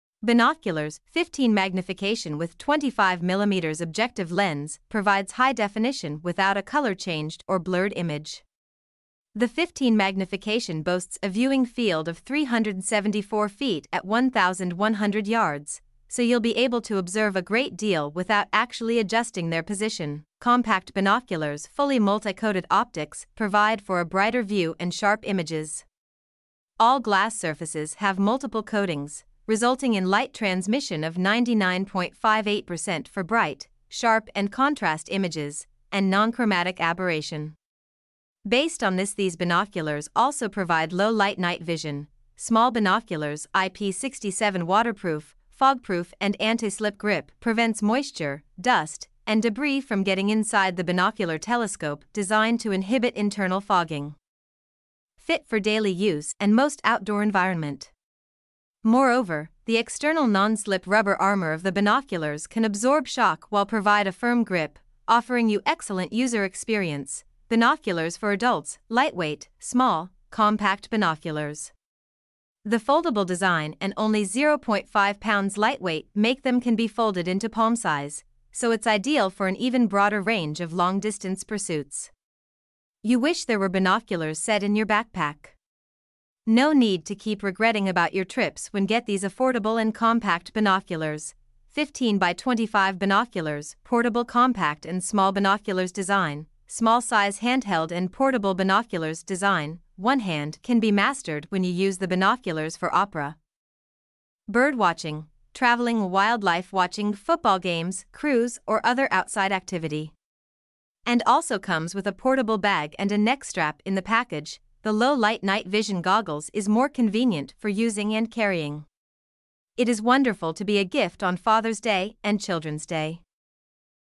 Binoculars 15x25 for Adults,Waterproof Binoculars with Low Light Night Vision - Rodcirant
Binoculars 15x25 for Adults,Waterproof Binoculars with Low Light Night Vision, Durable & Clear Binoculars for Sightseeing,Concerts and Bird Watching

*Webliven is an Amazon Associates Program Member.